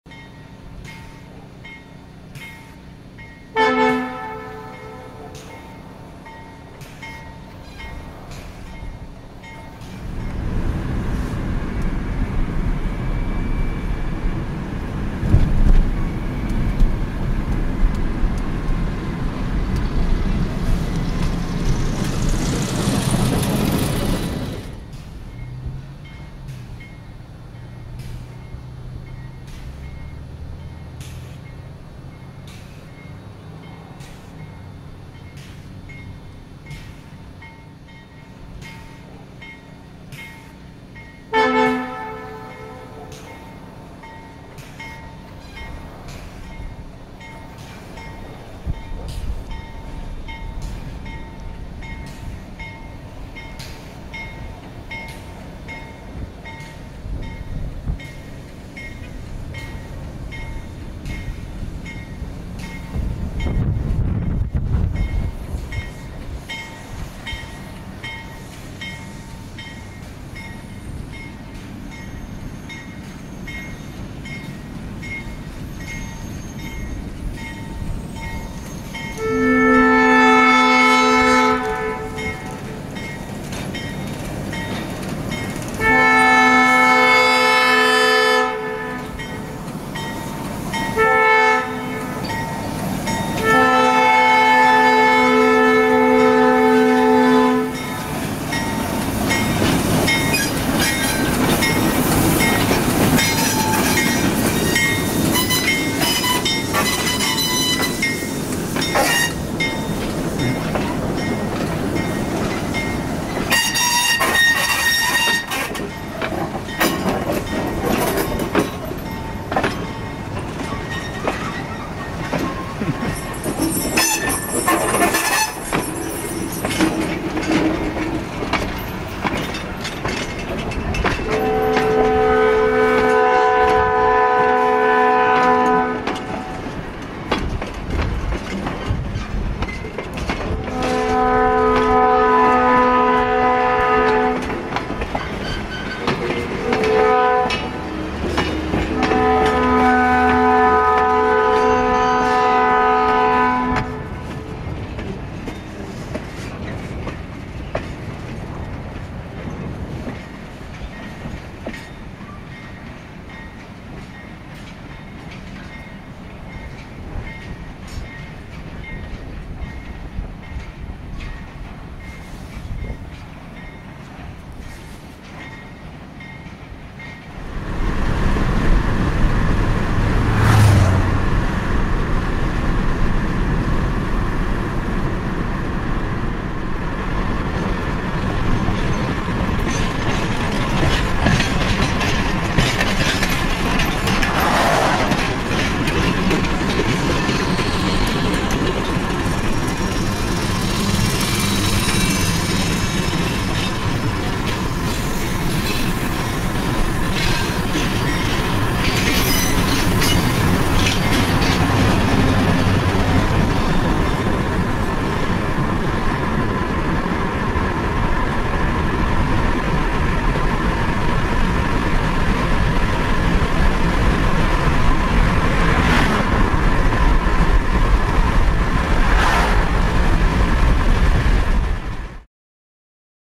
The 2008 Christmas Train in Connersville, Indiana
From December 13, 2008: This is the Christmas express departing Connersville for Metamora, Indiana.